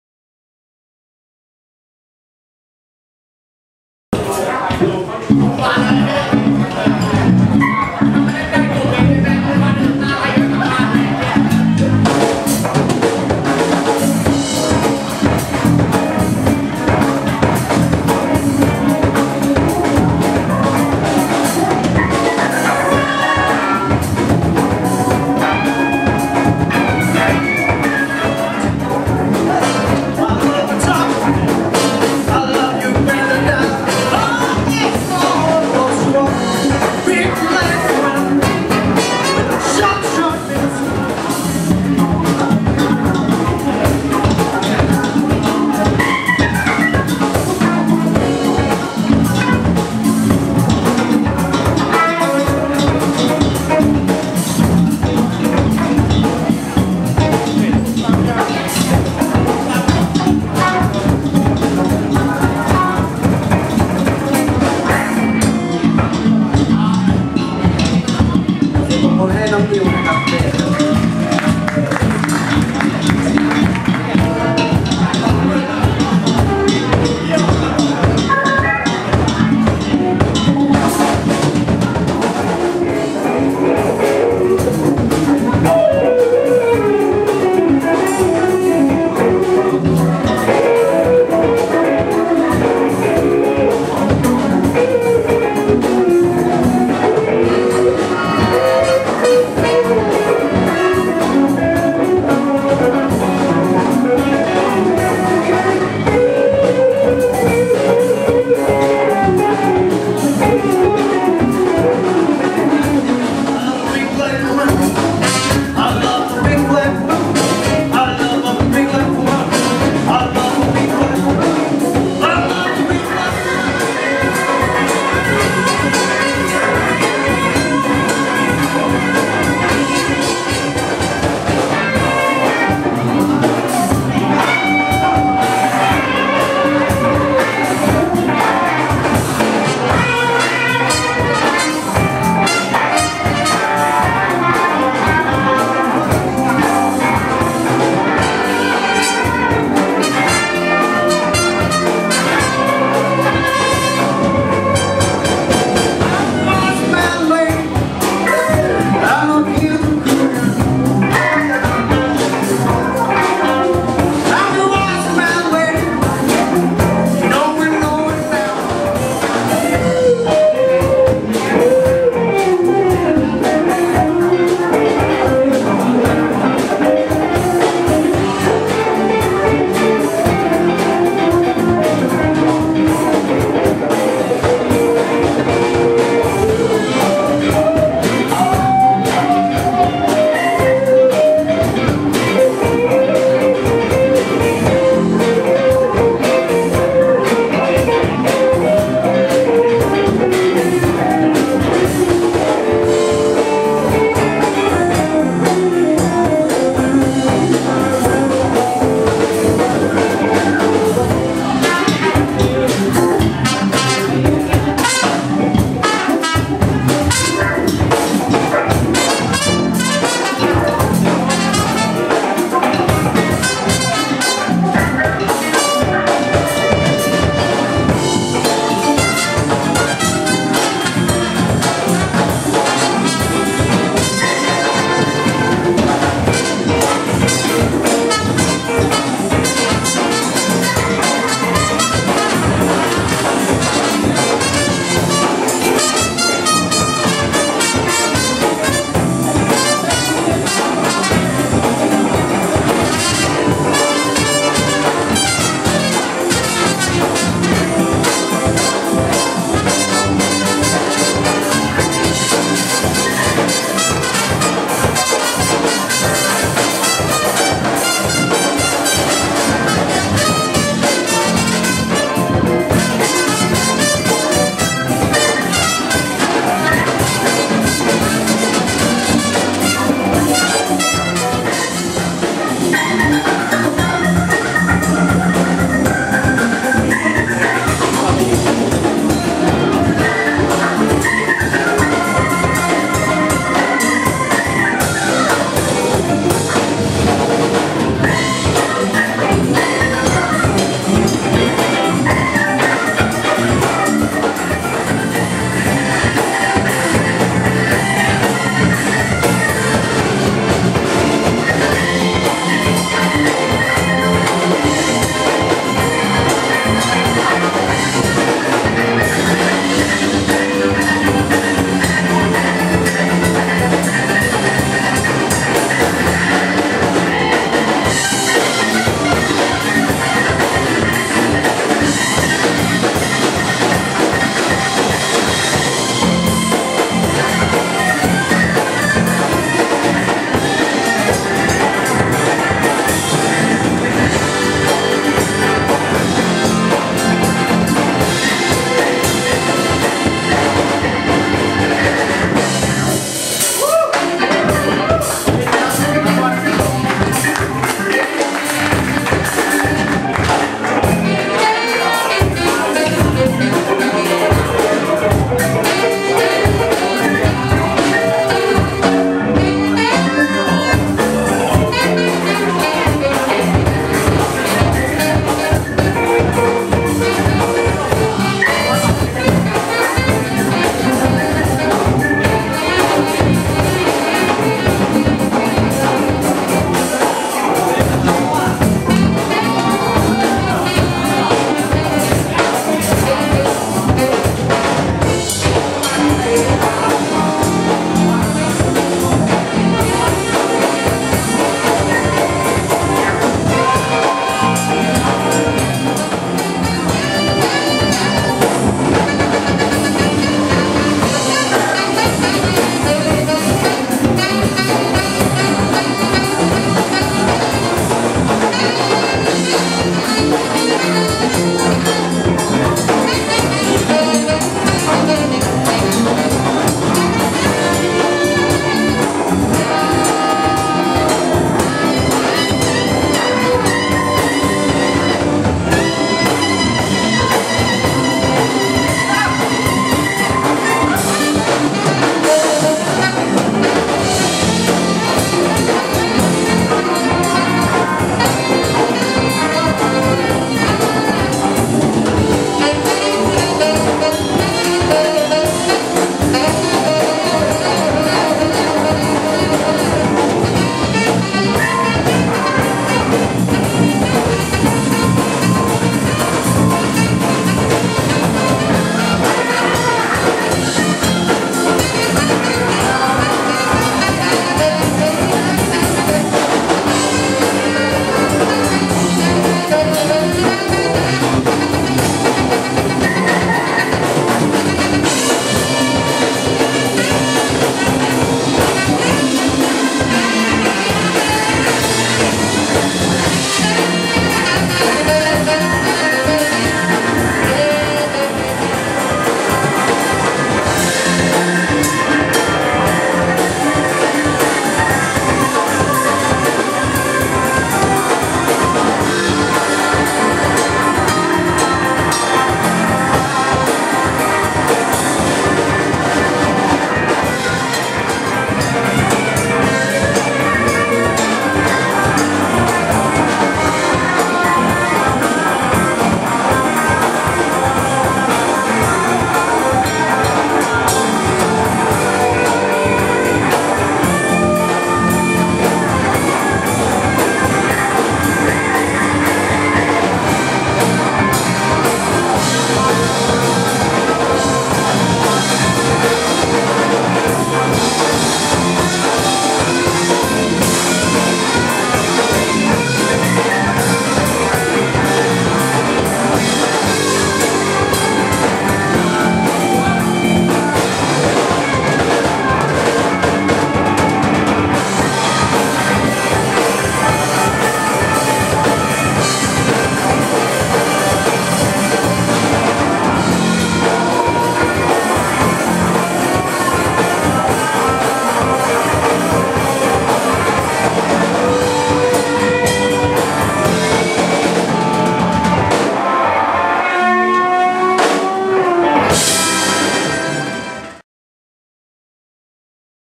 October Blues Fest. 2009 Funky Blues by Big Butter Beer
Bangkok Blues Club presents October Blues Fest. 2009 with funky blues by Big Butter Beer. October 24, 2009 at @ Pimo Pub.